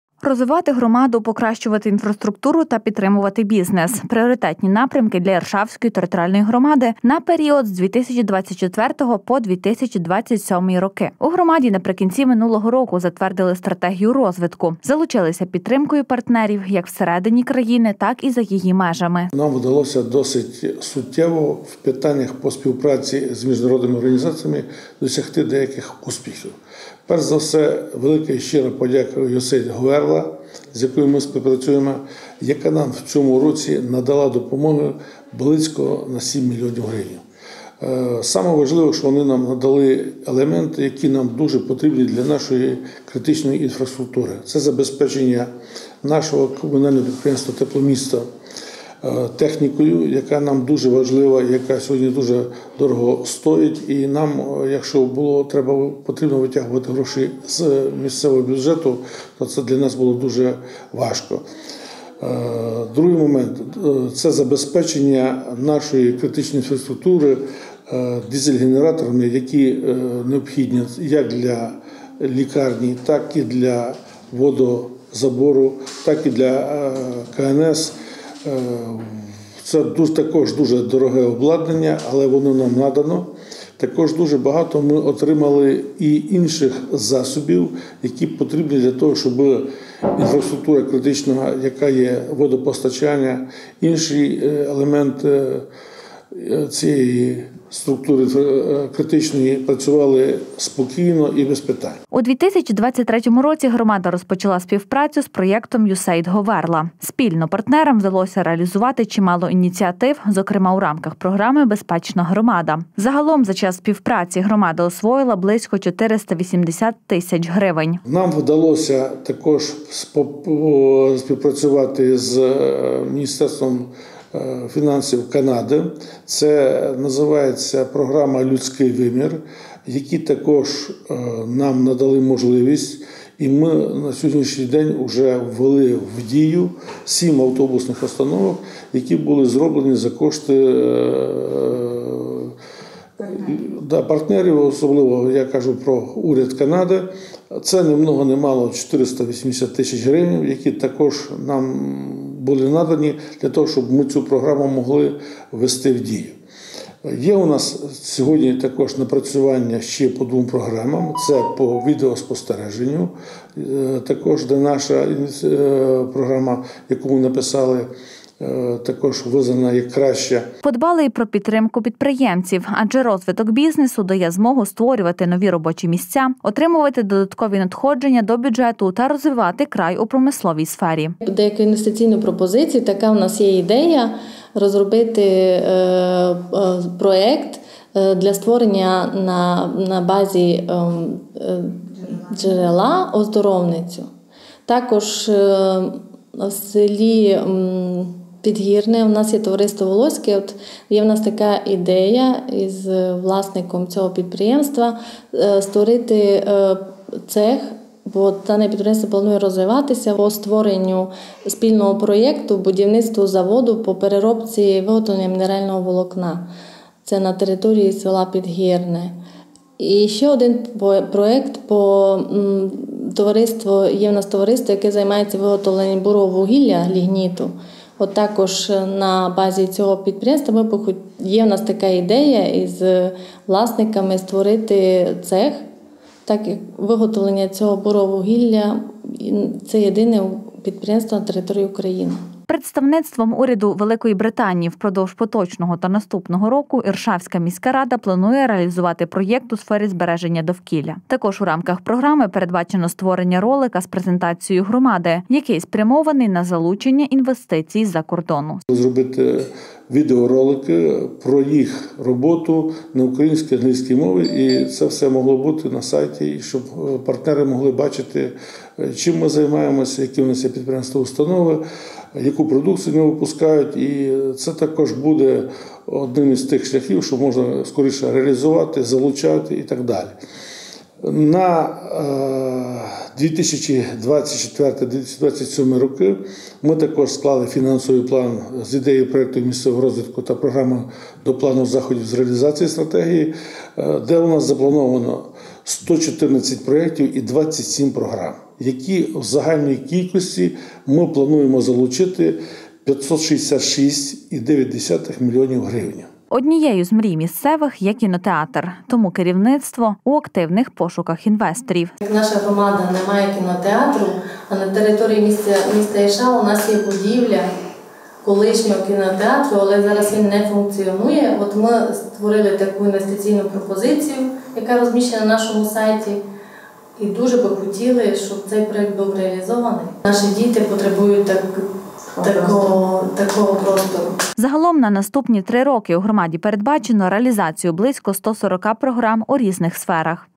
Розвиток громади
Розвиток громади , покращення  інфраструктуру та підтримка  бізнесу – пріоритетні напрямки для Іршавської ТГ на період з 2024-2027 року. У громаді наприкінці минулого року затвердили стратегію розвитку. Заручилися підтримкою партнерів,як в середині країни, так і за її межами.  У 2023 році громада розпочала співпрацю з проєктом USAID «ГОВЕРЛА», спільно партнерам вдалося реалізувати чимало ініціатив, зокрема у рамках програми «Безпечна громада».  Загалом за час співпраці, громада освоїла близько 480 тисяч гривень.  Подбали і про підприємців, адже розвиток бізнесу дає змогу створювати нові робочі місця, отримувати додаткові надходження до бюджету та розвивати край у промисловій сфері.  З представництвом уряду Великої Британії впродовж поточного та наступного року Іршавська міська рада планує реалізувати проєкт у сфері збереження довкілля. Також у рамках програми, передбачено створення ролика з презентацією громади, який спрямований на залучення інвестицій з-за кордону.  Однією з мрій місцевих  – є кінотеатр. Тому керівництво у активних пошуках інвесторів. Загалом на наступні 3 роки  у громаді передбачено реалізацію близько 140-ка програм у різних сферах.